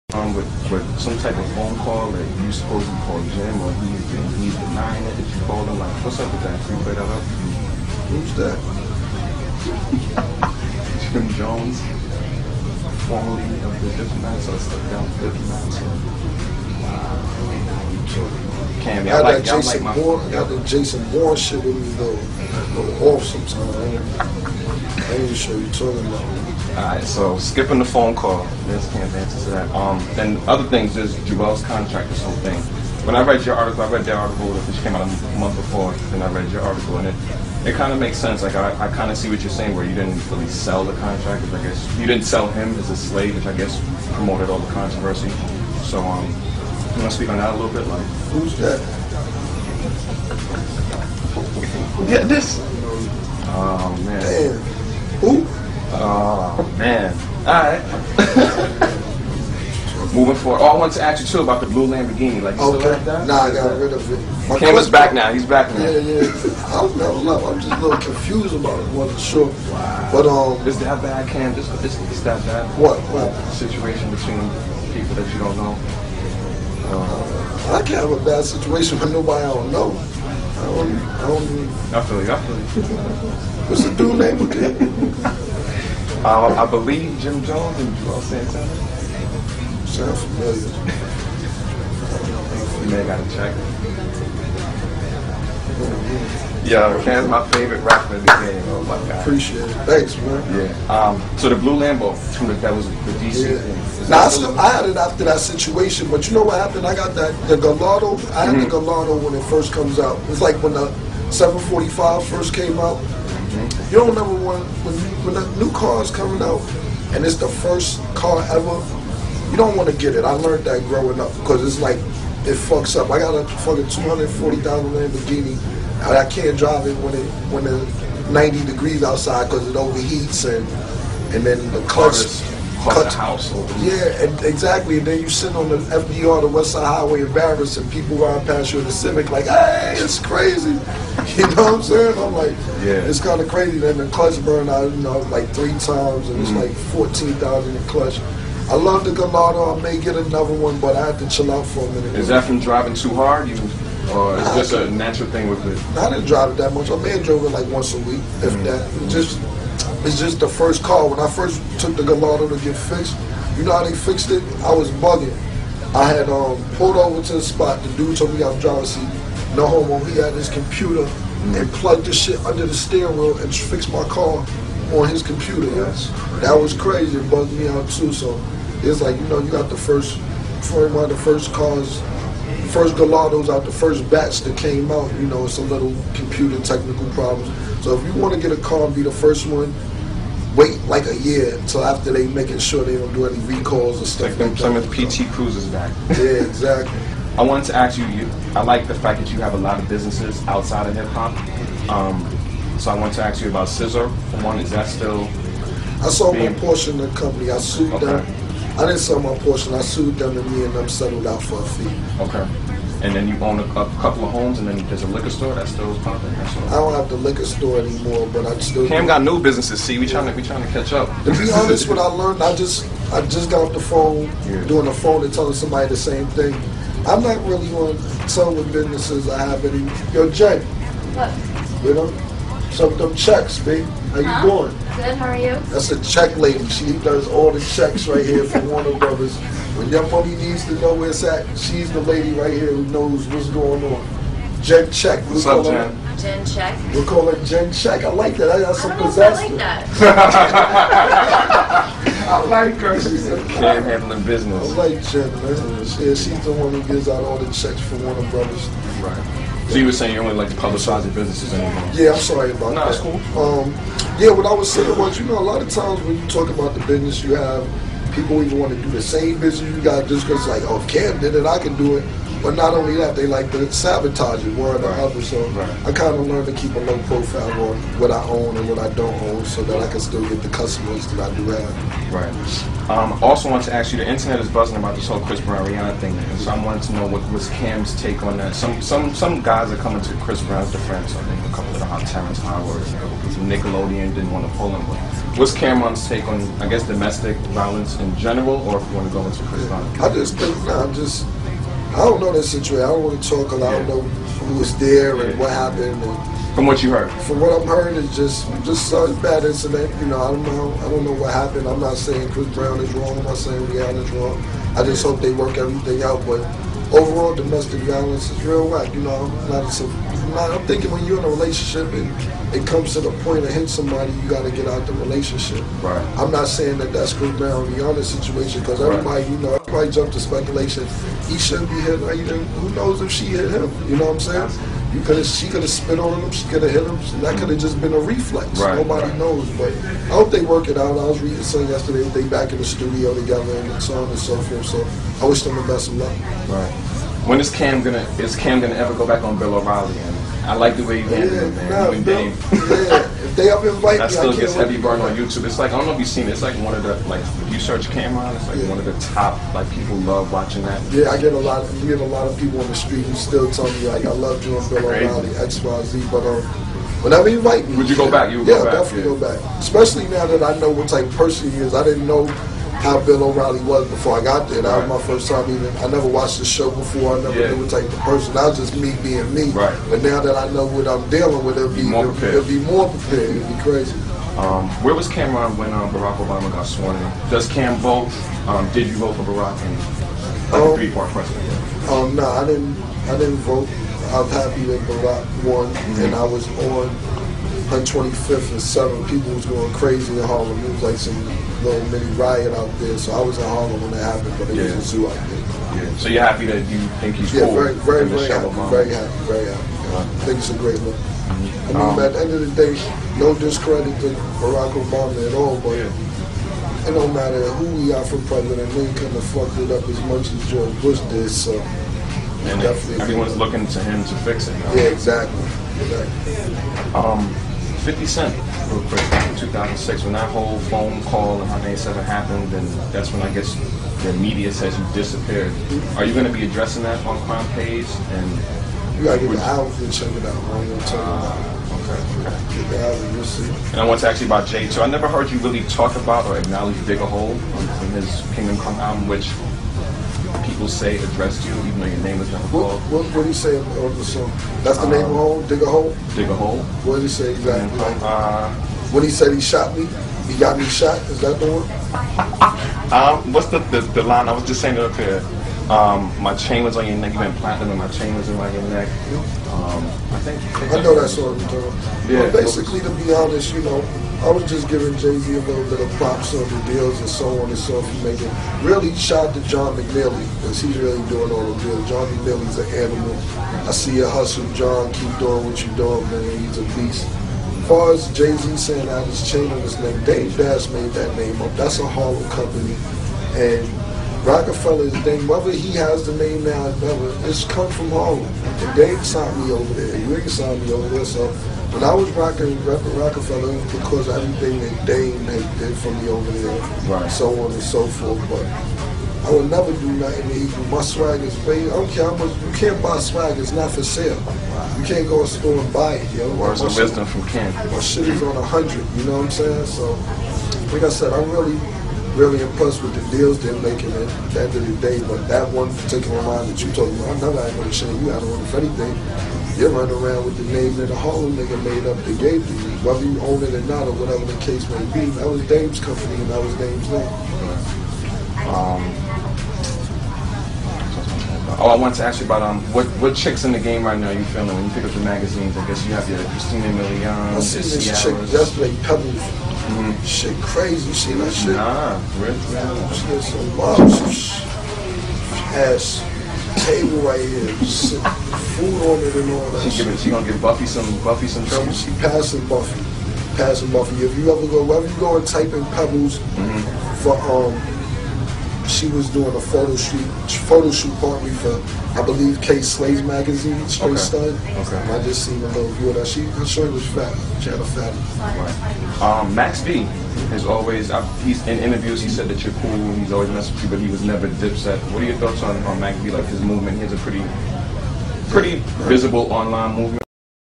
Cam ron Interview with TheLifeFiles Part 1
New Cam'ron interview...

Whats new for the album, the new Dipset...